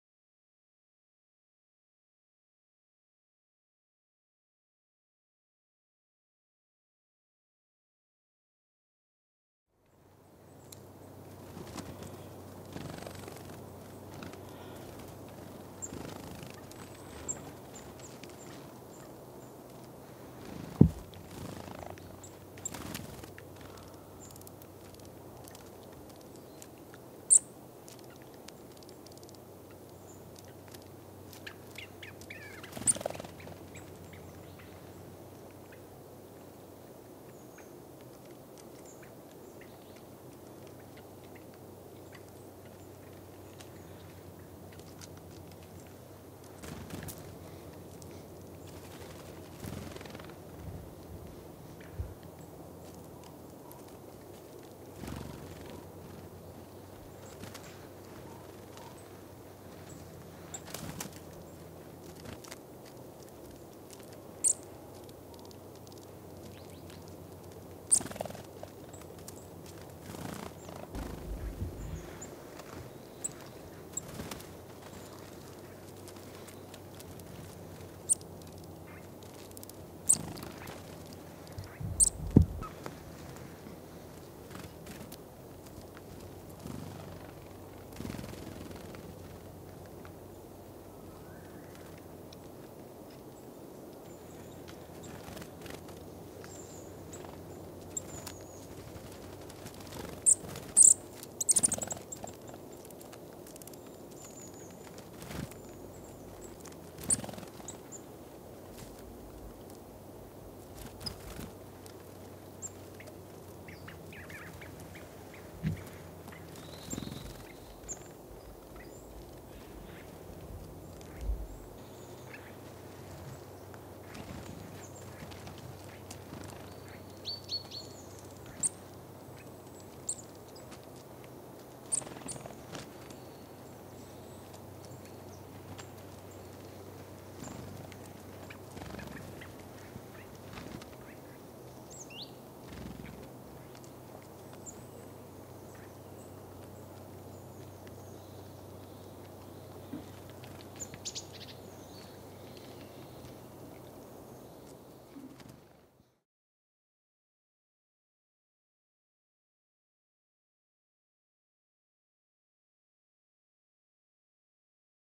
LAVSKRIKA Siberian Jay (Perisoreus infaustus) Klipp - 1478
Lavskrika.  Gästrikland 2012. (A-1). Siberian Jay in Sweden,  AROS FILM Fotograf: Gunnar Fernqvist. Västerås. Sweden.